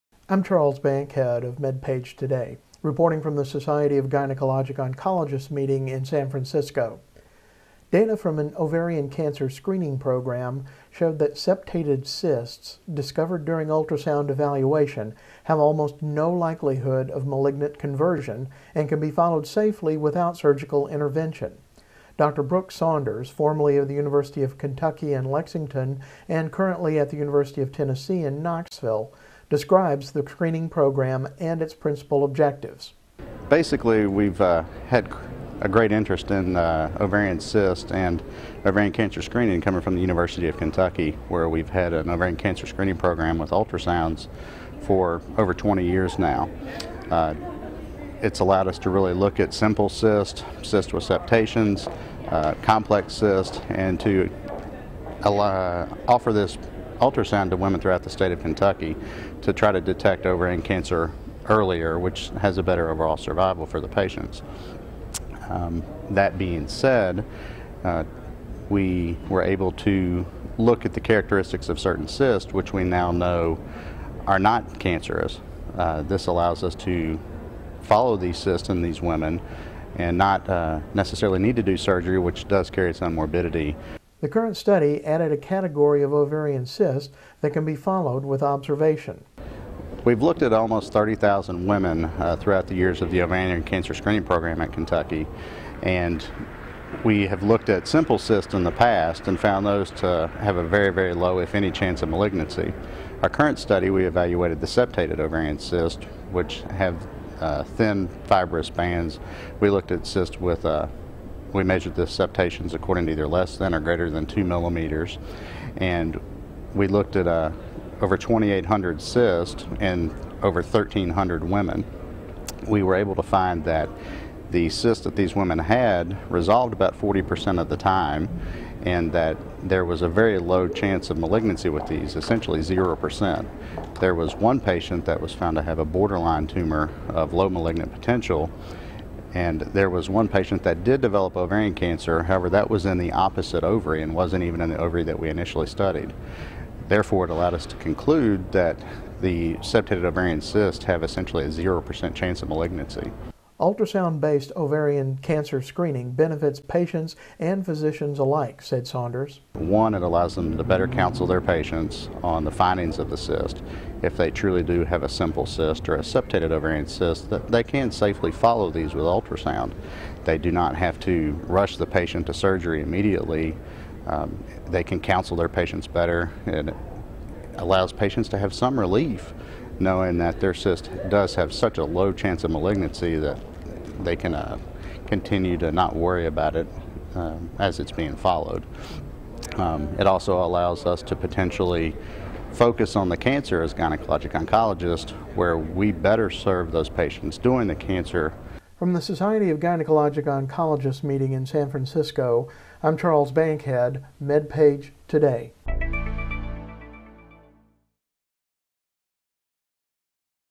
SGO: Low Cancer Risk with Septated Ovarian Tumors
SAN FRANCISCO -- Septated ovarian tumors have a low risk of malignancy and can be followed safely with ultrasound, data from a large prospective cohort study showed.

During a median follow-up of more than five years, one patient of 1,191 with persisting septated ovarian tumors developed ovarian cancer, and that lesion arose in the contralateral ovary.